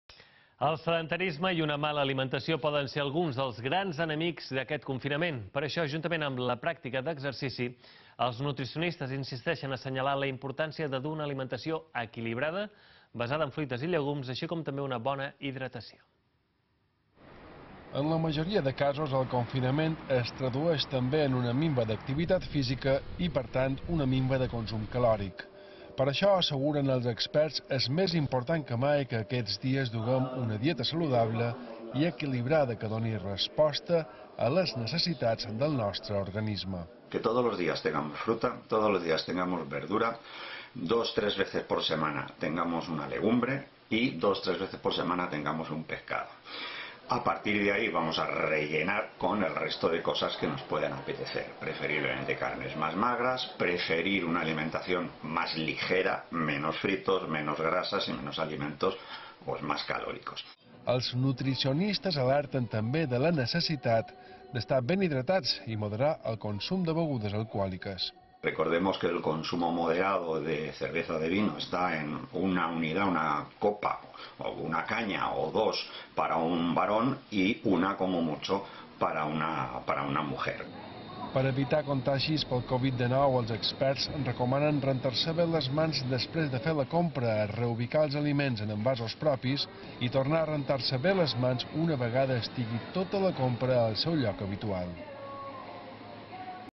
Els nutricionistes recomanen més que mai aquests dies una dieta equilbrada
En la majoria de casos, el confinament es tradueix també en minva d’activitat física i per tant en una minva també de consum calòric. Per això, asseguren els experts, ara és més important que mai mantenir una dieta saludable i equilibrada que doni resposta a les necessitats reals del nostre organisme. Els nutricionistes alerten de la necessitat d’estar ben hidratats i moderar el consum de begudes alcohòliques.

D’altra banda i per evitar possibles contagis del COVID-19 recorden que és molt important prendre mesures higièniques a l’hora d’anar a fer la compra. Rentar-se be les mans en arriabar a casa, rentar o dipositar cada un dels productes comprats en envasos propis i tornar a rentar-se les mans una vegada hem col·locat la compra al seu lloc són algunes de les mesures més efectives.